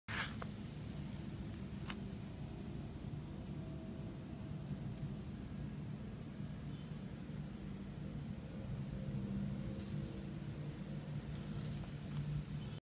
The headless grass hopper
This video is about an alive headless grasshopper. I hope you enjoyed this video.